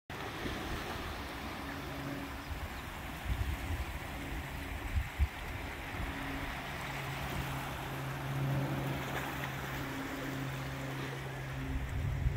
Clear waters of West side Maui.
Views of the water and sky alond West Side Maui.